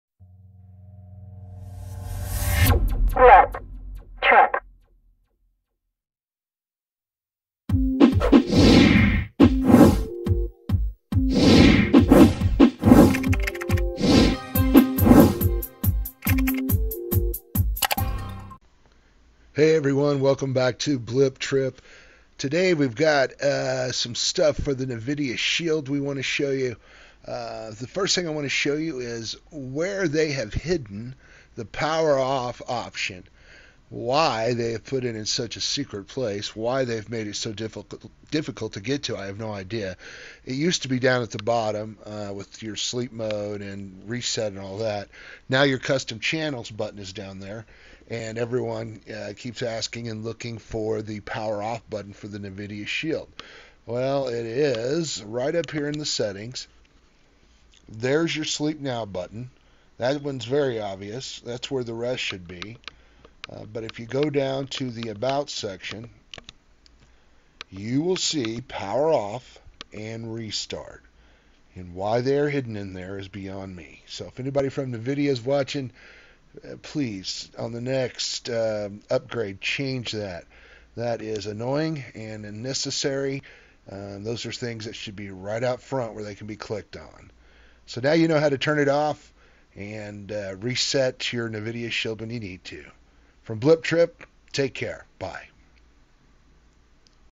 How to power off your Nvidia Shield
After the OREO update, where did the hide the power off and reset button? Find out here.